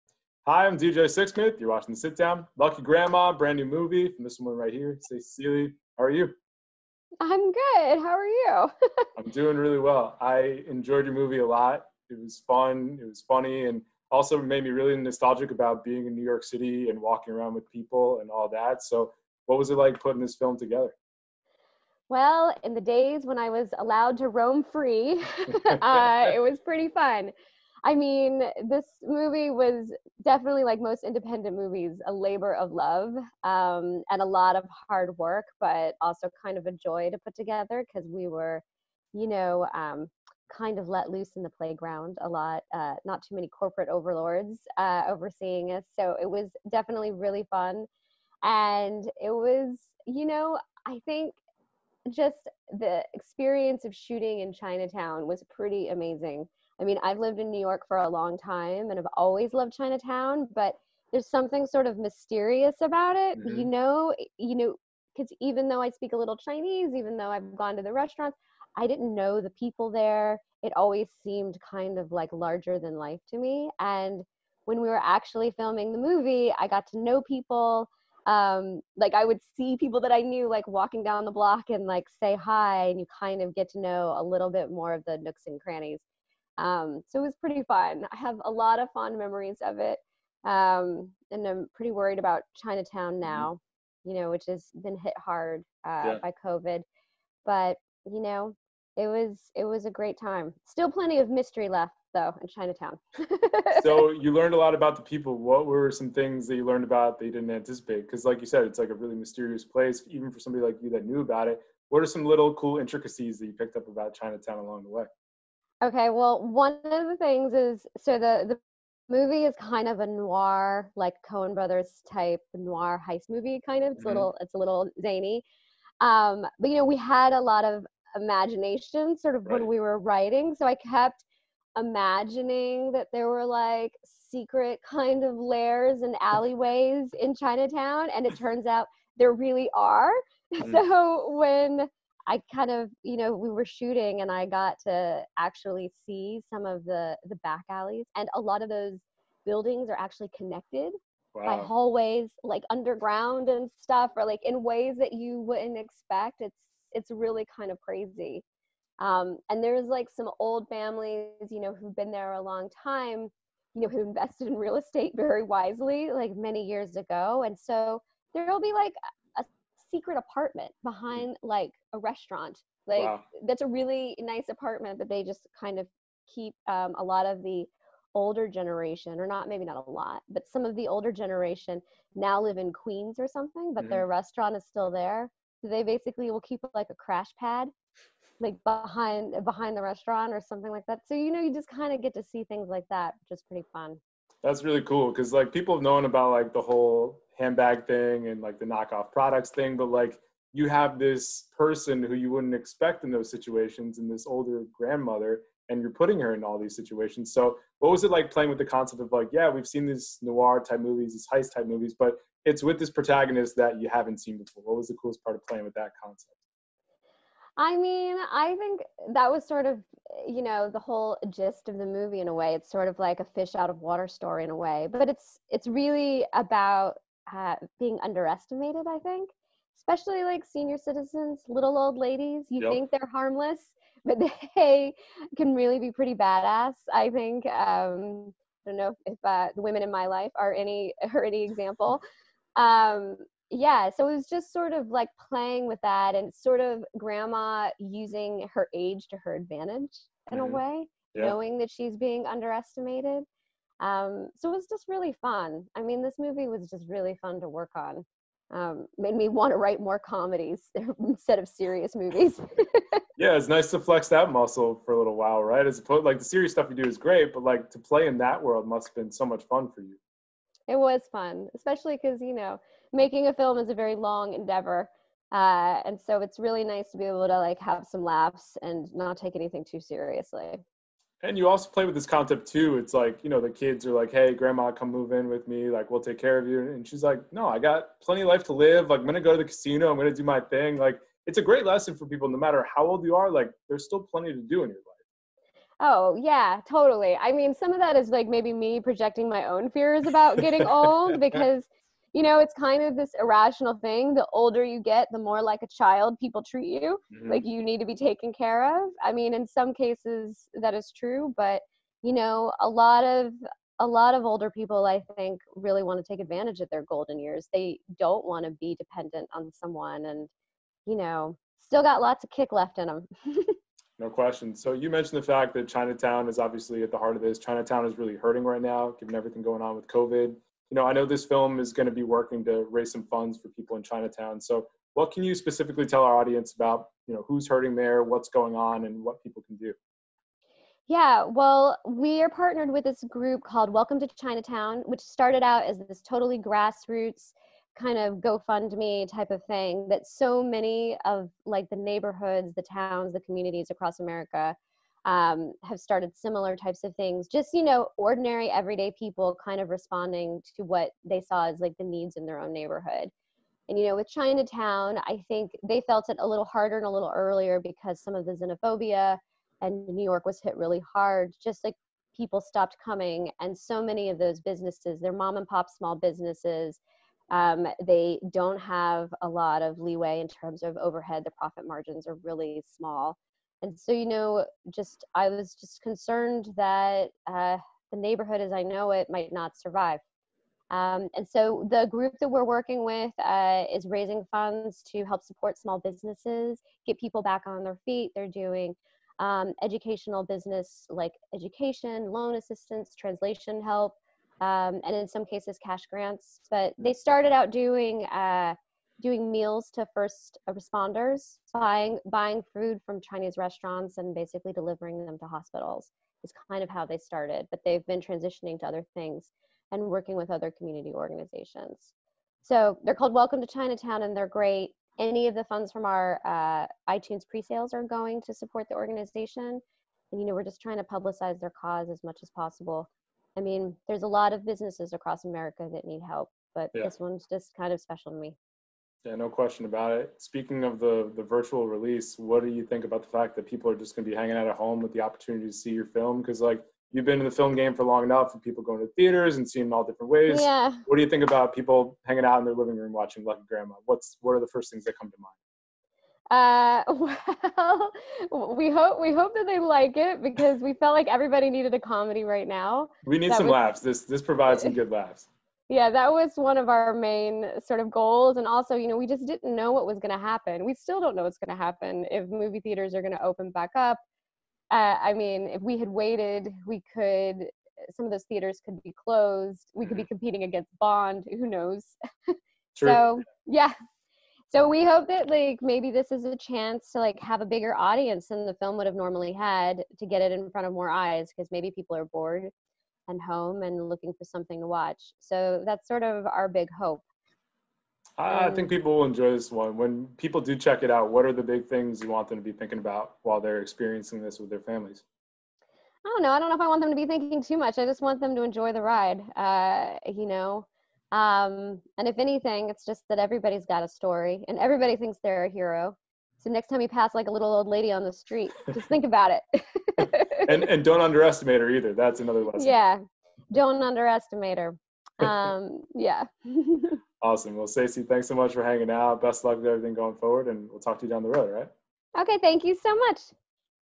Director Sasie Sealy On "Lucky Grandma" & Chinatown
The director talks with CBS Local's DJ Sixsmith about her new money heist movie with a grandma as the protagonist, the importance of Chinatown to New York, and making movies in 2020.